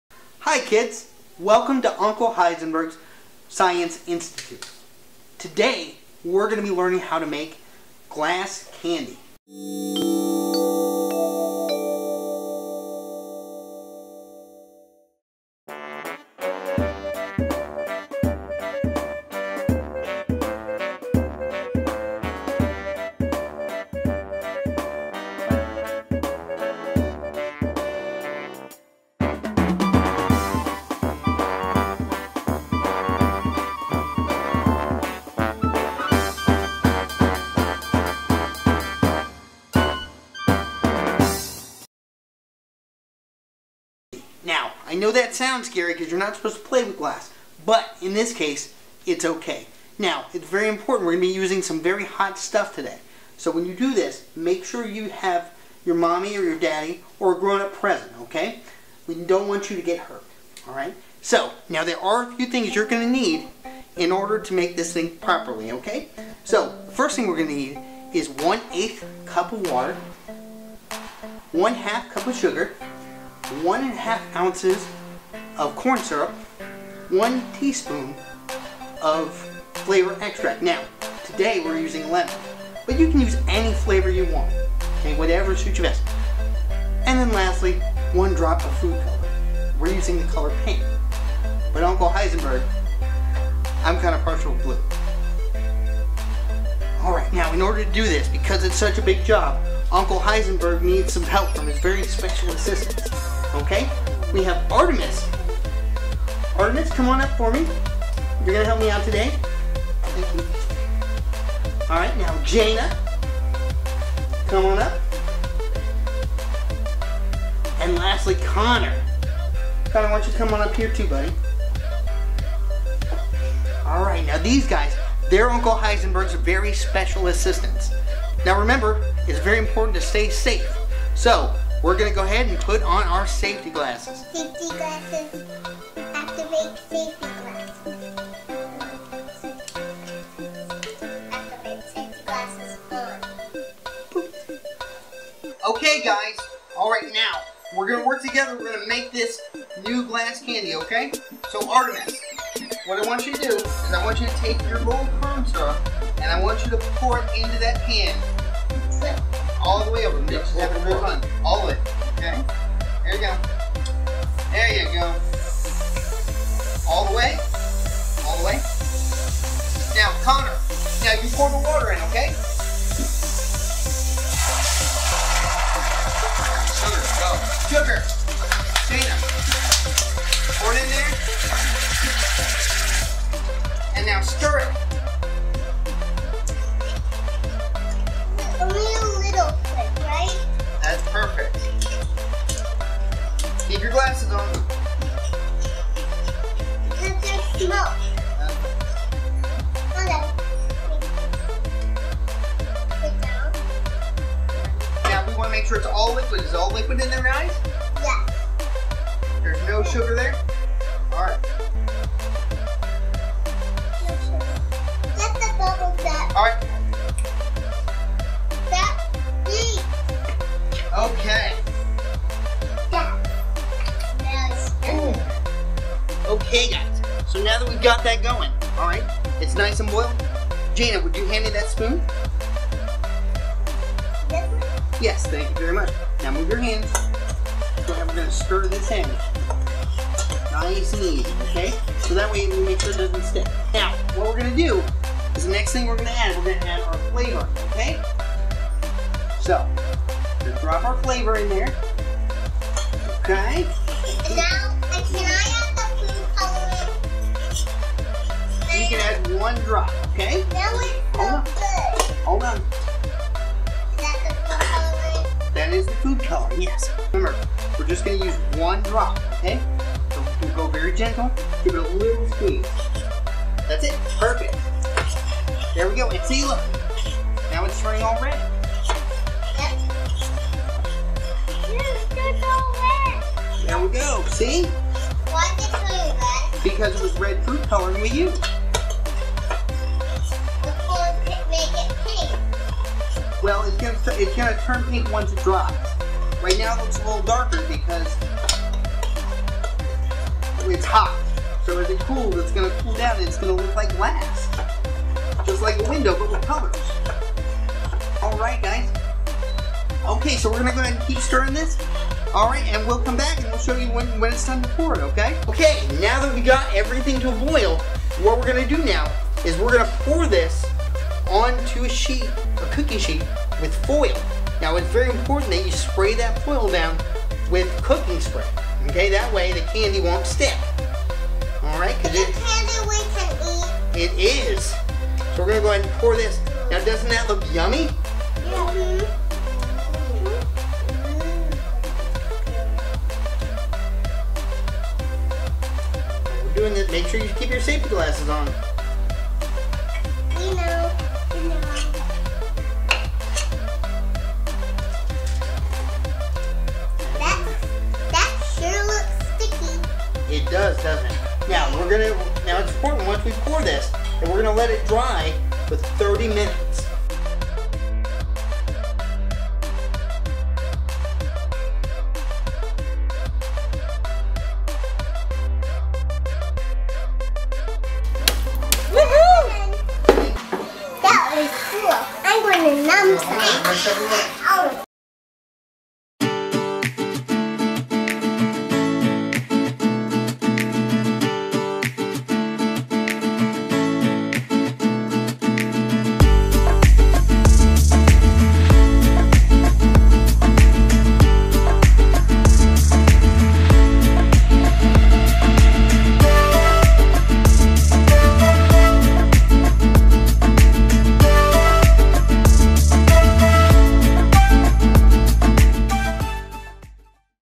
Uncle Heisenberg's Science Institute Presents: Glass Candy
Uncle Heisenberg and the gang are here with their FIRST EPISODE! In this episode they are going to show you how to make glass candy. Remember kids, ALWAYS do your experiments safely... that means wear the proper equipment and ALWAYS have a grown up help you!